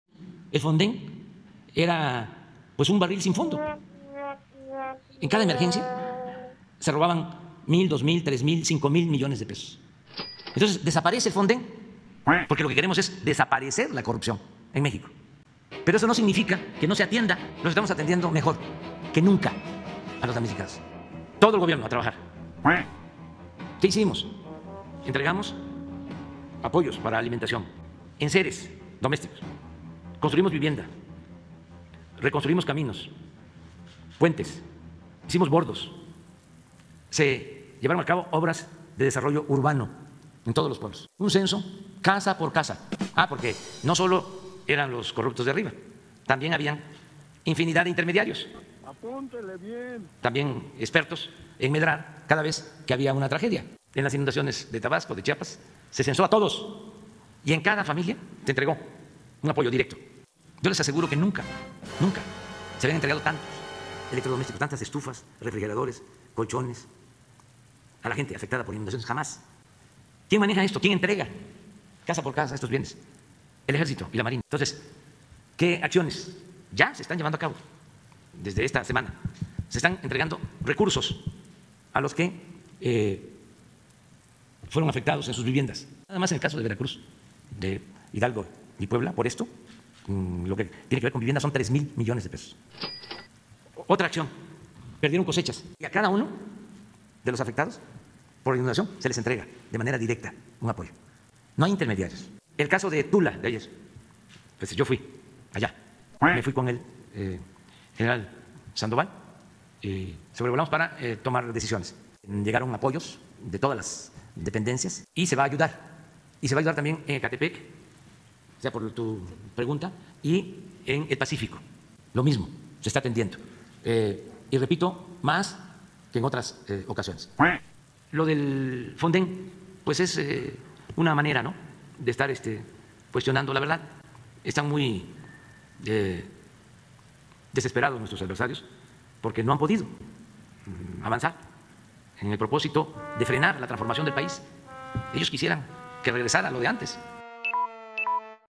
El FONDEN era un barril sin fondo, por eso desapareció, para desaparecer la corrupción en México 🦆
El FONDEN era un barril sin fondo, por eso desapareció, para desaparecer la corrupción en México “ahora se está ayudando como nunca”🦆